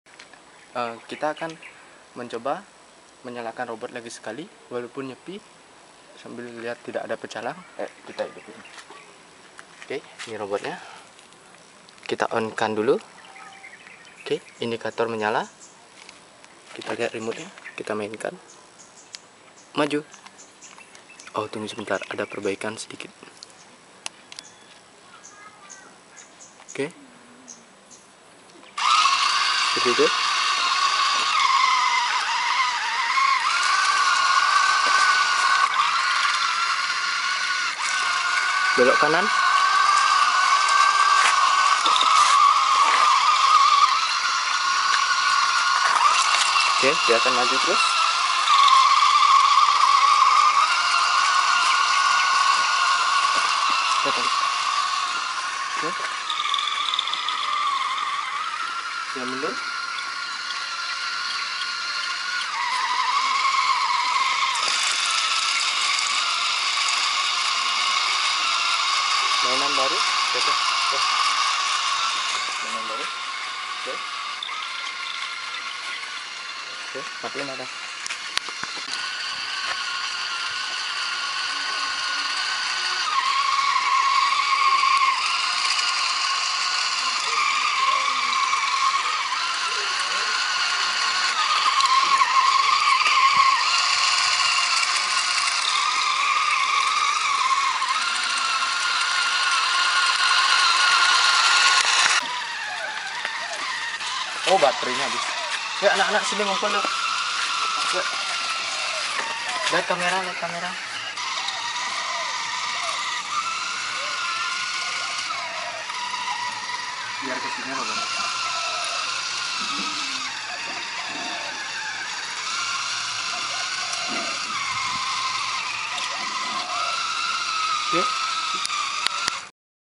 Kontrol Robot dengan Wireless Stick PS2 + Coding
project ini merupakan project yang sudah lama menjadi keinginan ku merealisasikannya. bermodal stick PS2 wireless dan sebuah board Erulduino pada saat libur hari raya nyepi aku mencoba merancangnya. saksikan aksinya kawan di video ini!